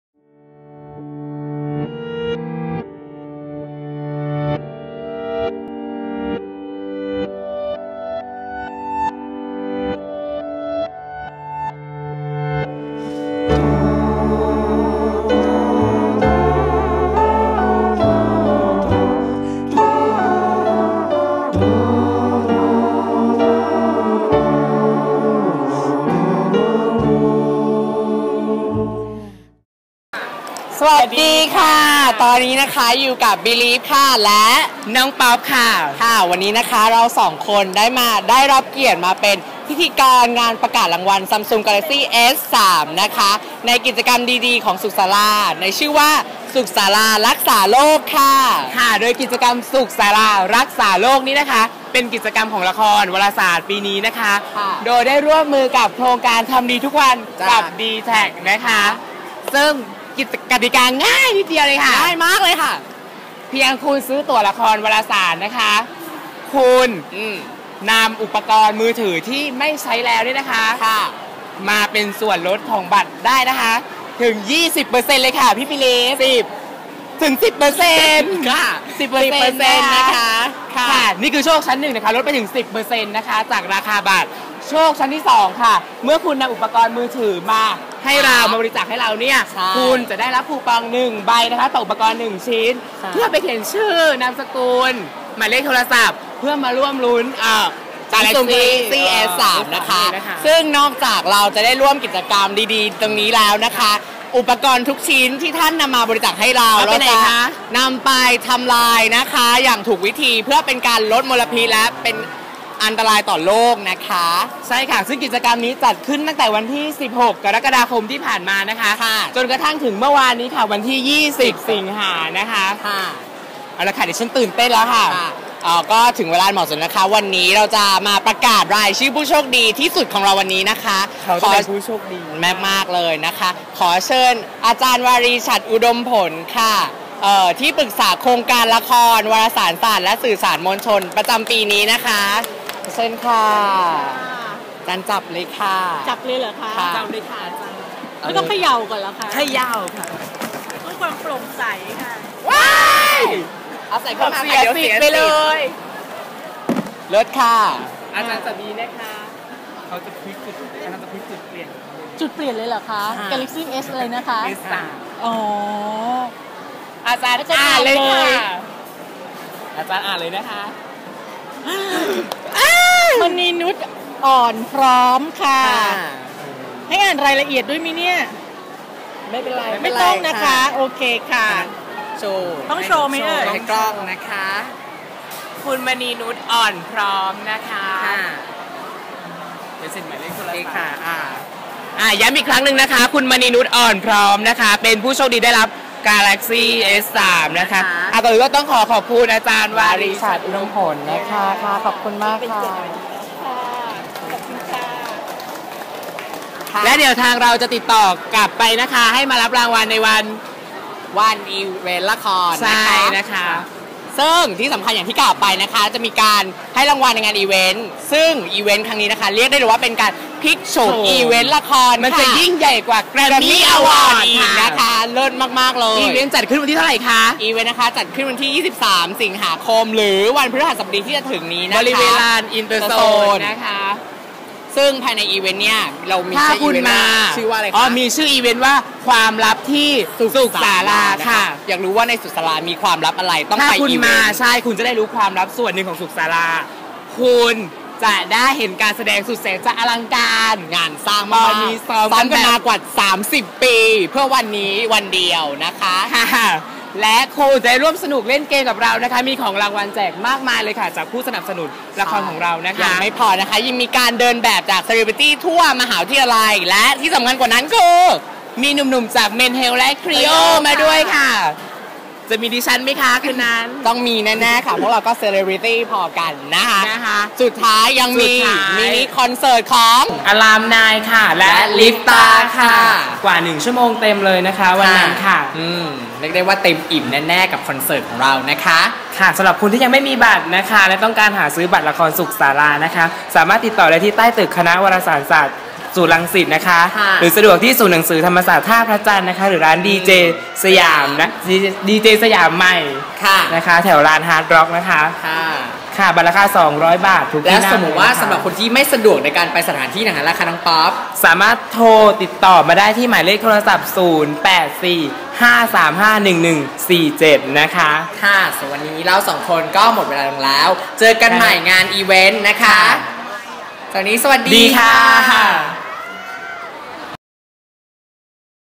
ผู้โชคดี กับ สุขศาลา รักษาโลก
งานประกาศผลรางวัล กับกิจกรรม สุขศาลา รักษาโลก
โดยละคอนเวทีคณะวารสารศาสตร์และสื่อสารมวลชน มหาวิทยาลัยธรรมศาสตร์ ร่วมกับ โครงการทำดีทุกวันกับดีแทค จัดกิจกรรมดีๆ เพื่อช่วยกันรักษาโลกอันเป็นที่รักของเรา

กติกาง่ายๆ เพียงแค่ท่านซื้อบัตรละคอนเวทีวารสารฯ และนำอุปกรณ์มือถือที่ไม่ใช้แล้วมาบริจาค ท่านก็จะได้รับคูปองชิงโชค Samsung Galaxy SIII

และผู้โชคดีในกิจกรรมนี้คือ คุณ มณีนุช อ่อนพร้อม!

ขอแสดงความยินดีกับผู้โชคดีด้วยครับ ทางทีมงานจะติดต่อคุณมณีนุช เพื่อมารับรางวัลในงานเปิดตัวละคอนเวทีวารสารฯ ประจำปี 2555 เรื่อง สุขศาลา อย่างเป็นทางการใน วันที่ 23 สิงหาคม พ.ศ. 2555 ที่ลานกิจกรรมอินเตอร์โซน มหาวิทยาลัยธรรมศาสตร์ ศูนย์รังสิต